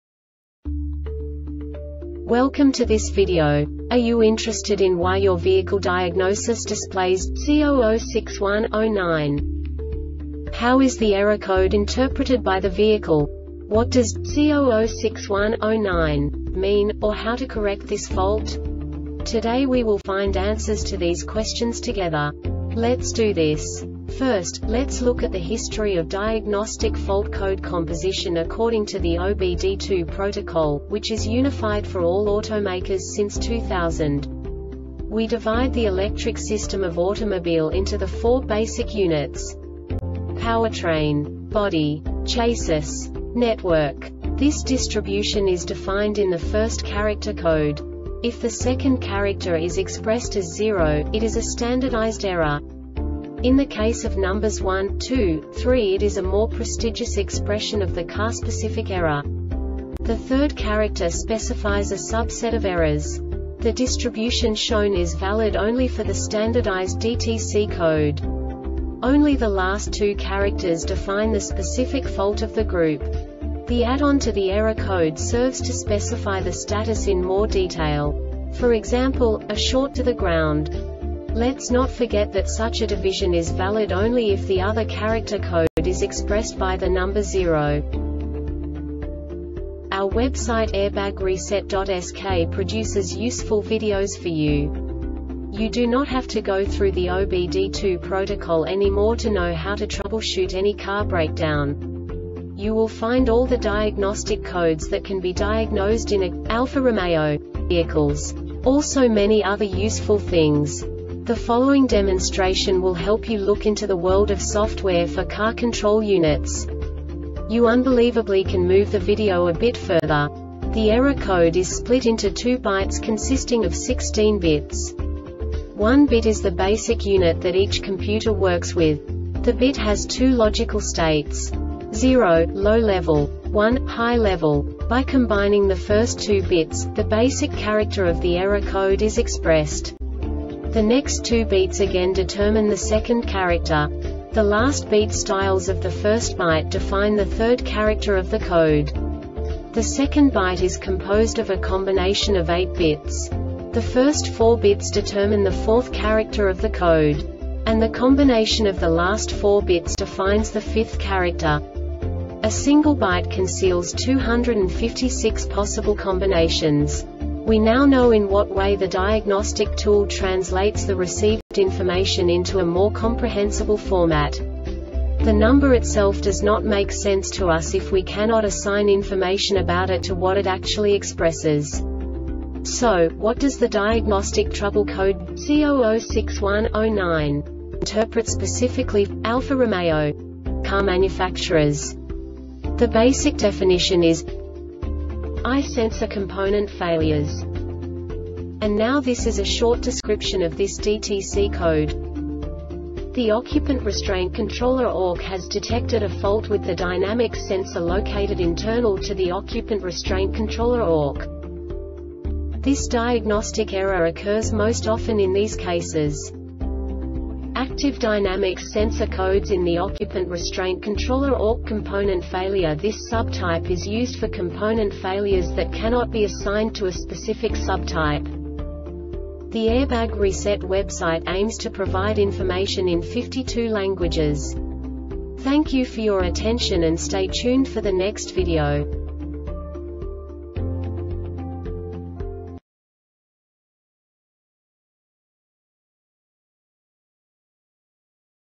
DTC Alfa-Romeo C0061-09 Short Explanation
Contents:
0:21 Basic DTC analysis according to OBD2 protocol standard.
1:48 Insight into programming
2:58 A diagnostically understandable description of the Diagnostic Trouble Code
3:05 Basic error definition

Make:
Alfa Romeo
Code:
C0061-09
Definition:
Ay Sensor-Component Failures
Description:
The Occupant Restraint Controller (ORC) has detected a fault with the Dynamics Sensor located internal to the Occupant Restraint Controller (ORC).
Cause:
Active Dynamics Sensor Code(S) In The Occupant Restraint Controller (Orc)
Failure Type:
Component Failure